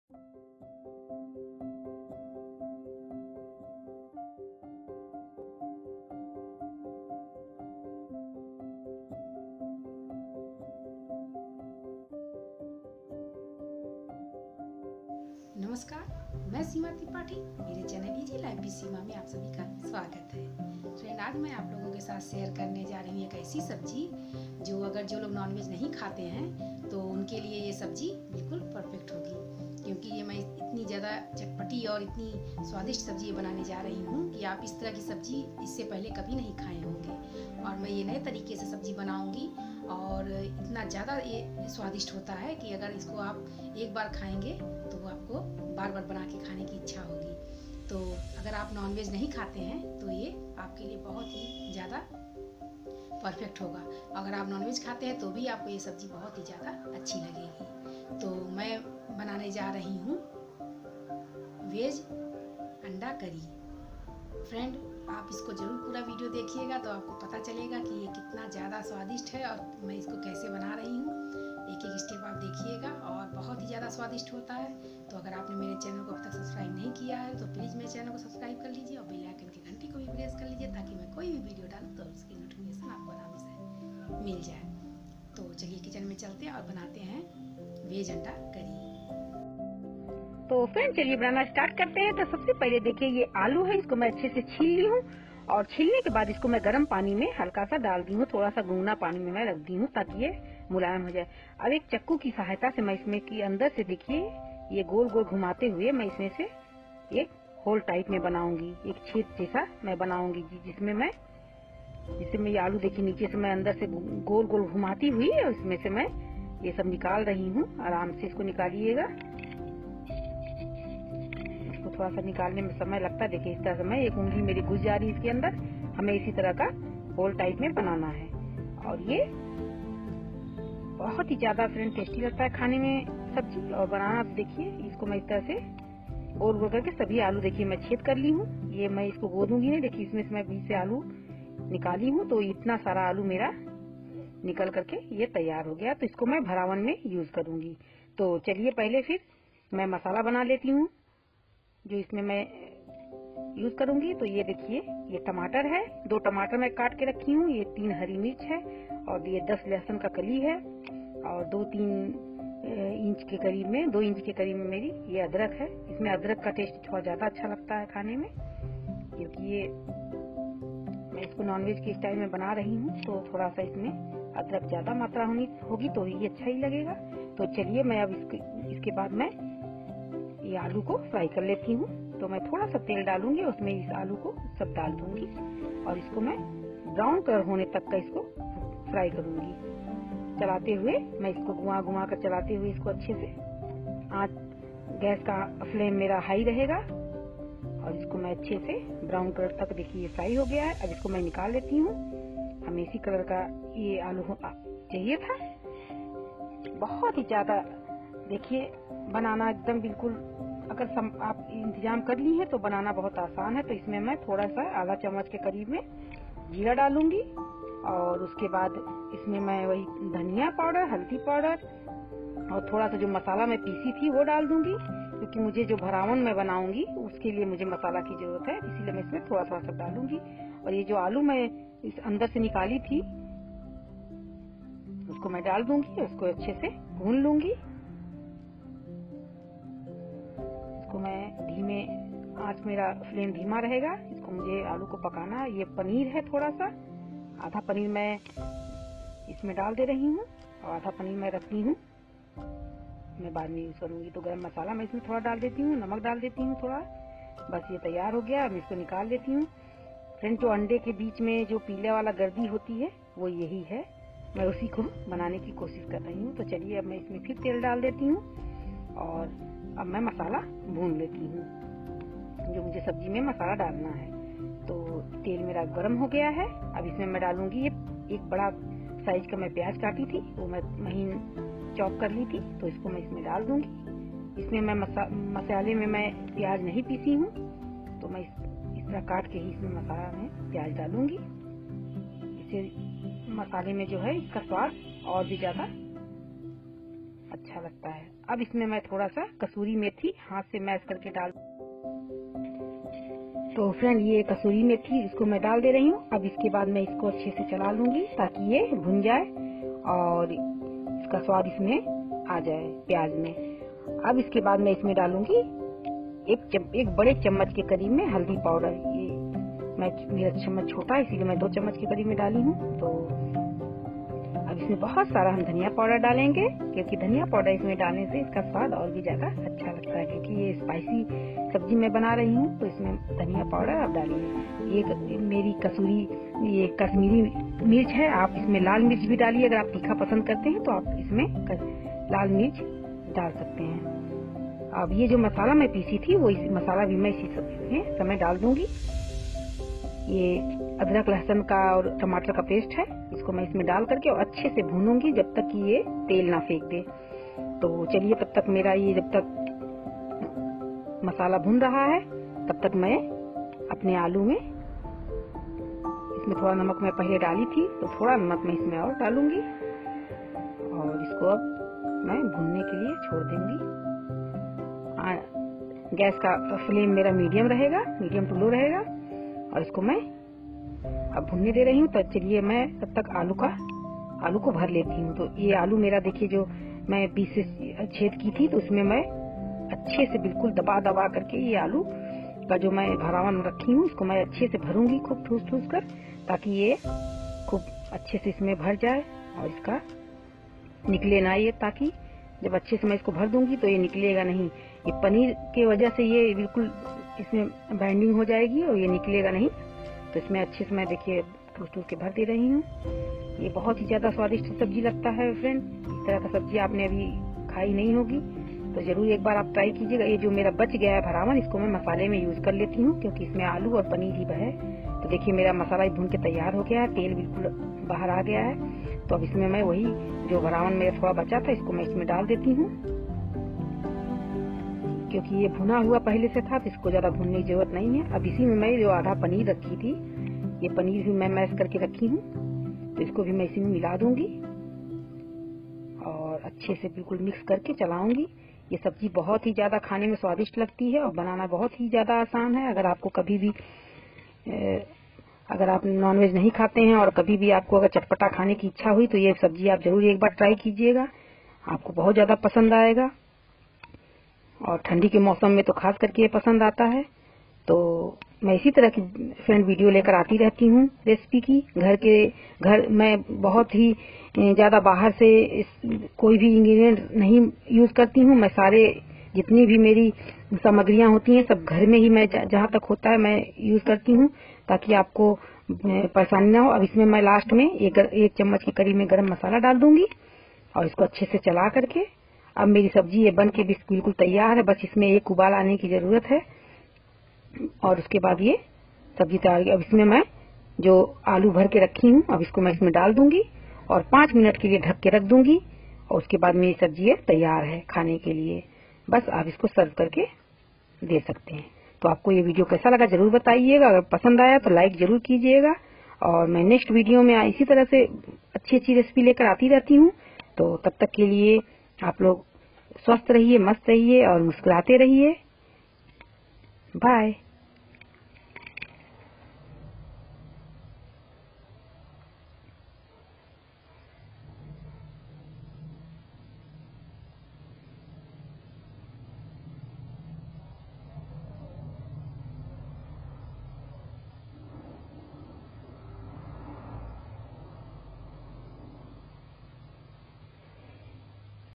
Veg Anda Curry Recipe .Vegetarian Logon ke Liye Ek Swadisht Sabji ek Bar JarurTrai Karen.
vegandacurryrecipe dumaloorecipe easy lifewithSeema#Kashmiridumaloof#aaluki s
sabji#KashmiridumalooHindi#hello  friends main Seema Tripathi mere channel easy life with Seema mein aap sabhi ka swagat hai aaj mein aaloo ki a kaisi sabji banaa rahi hoon jiska khane ke bad aap Anda khana bhul jaenge yah sabji Ek Bar Apne kitchen main jarur aur mere channel ko subscribe Kar lijiyeaur video Pasand Aaye to ek like kijiye thank you so much